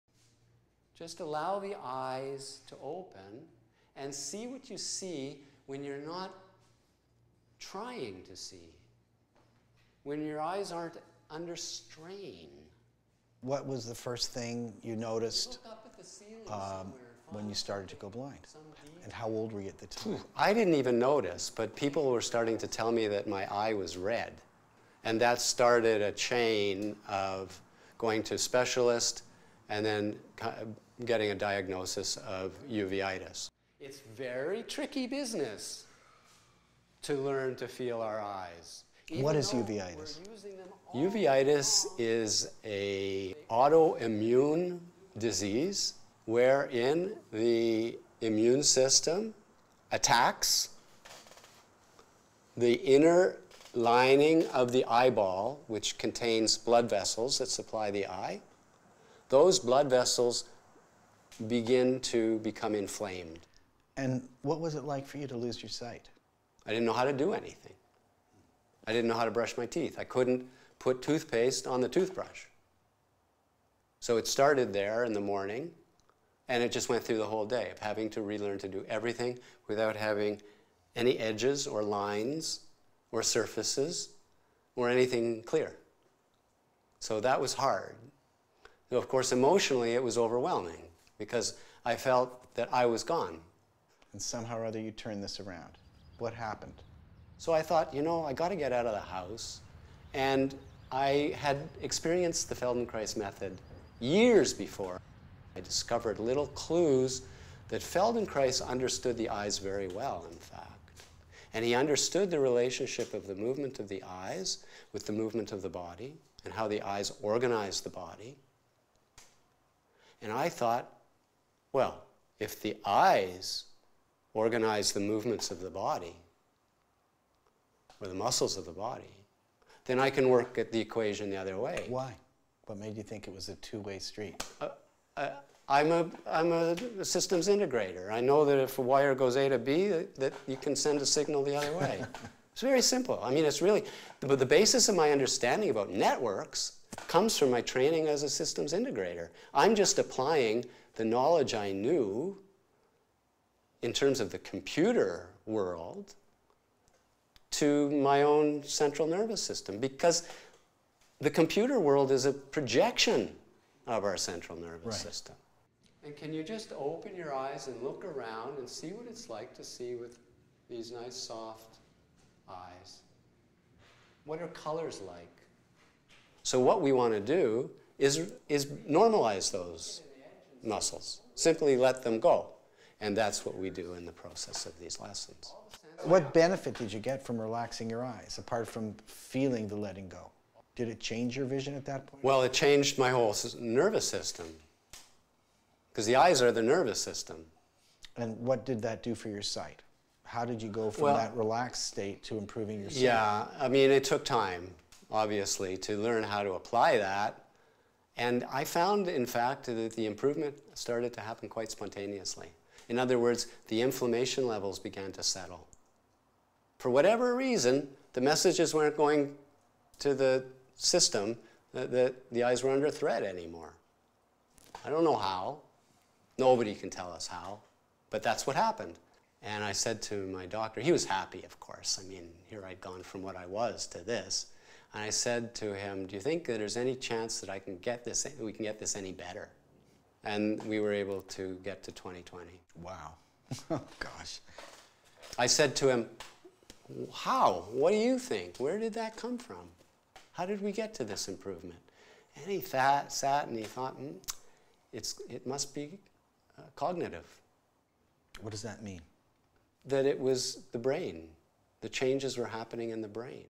Uveitis robbed David Webber of his vision. This is how he recovered it after doctors gave up hope.
Seeing Clearly was designed to help others improve their eyesight, decrease the symptoms of uveitis, reduce the symptoms of computer vision syndrome, lessen their dependence on glasses, and reduce jaw tension.

Norman Doidge, MD, interviews David Webber.  Video courtesy of 90th Parallel Productions.